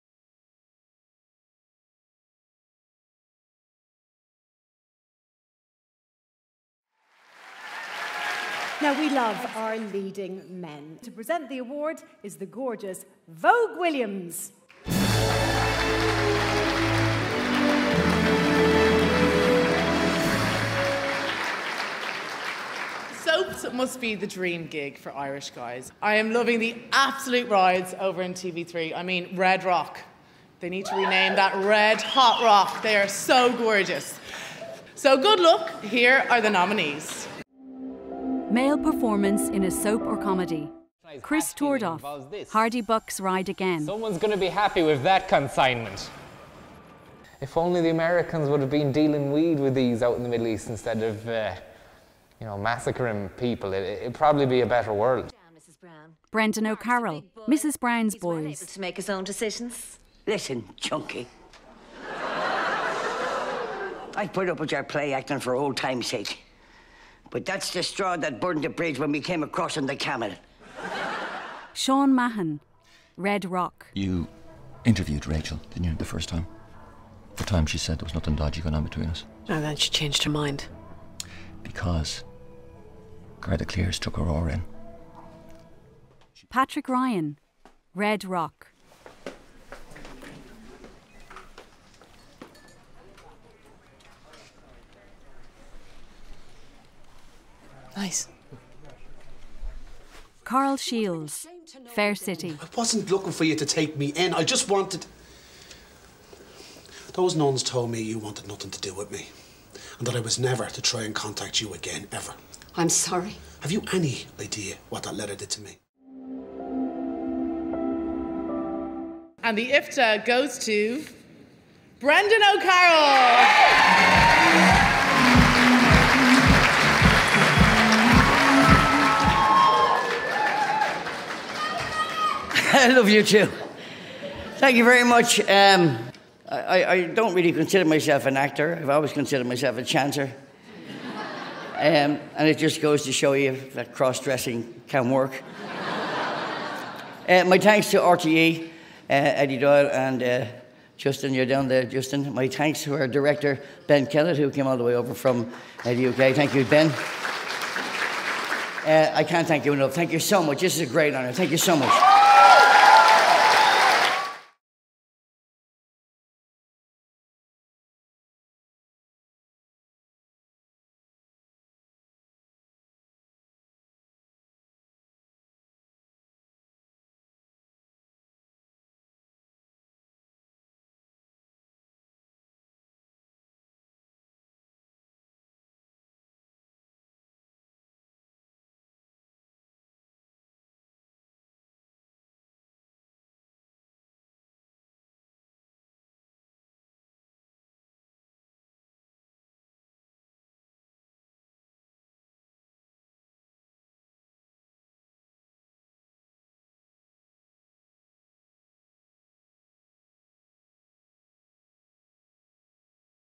Brendan O'Carroll - Winner of Best Male Performance Soap/Comedy, IFTA Gala TV Awards 2016
Brendan O'Carroll accepts the IFTA for Best Male Performance Soap/Comedy for his role in Mrs. Brown's Boys, presented by Vogue Williams